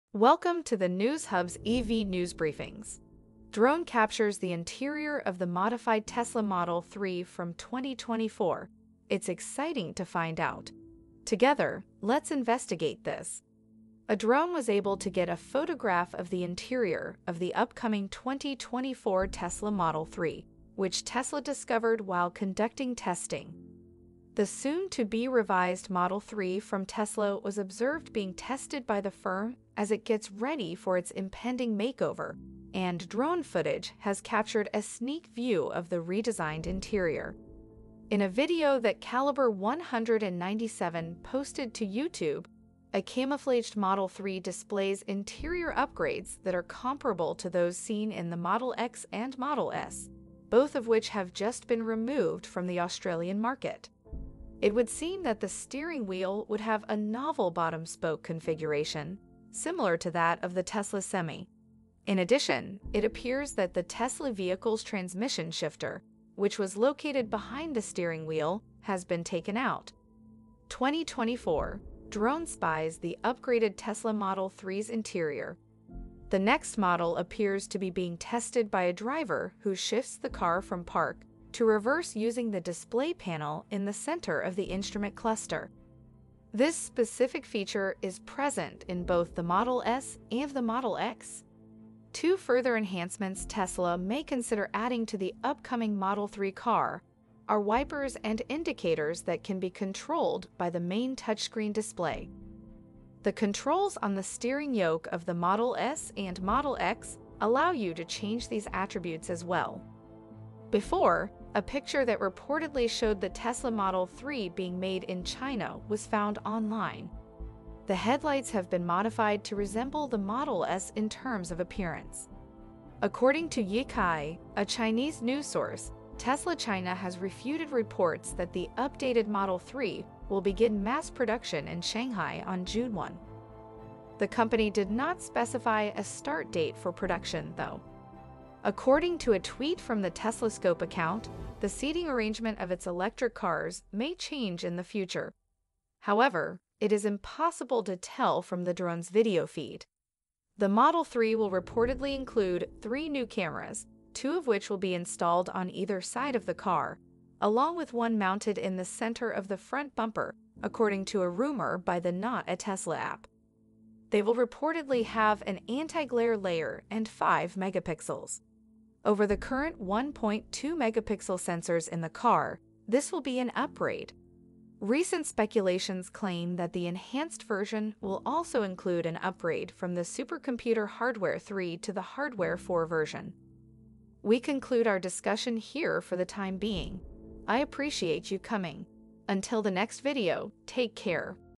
Drone spies 2024 Tesla Model 3's updated interior
Green Energy Innovations, Tesla News, Cutting Edge Updates In The EV and Transport Industry.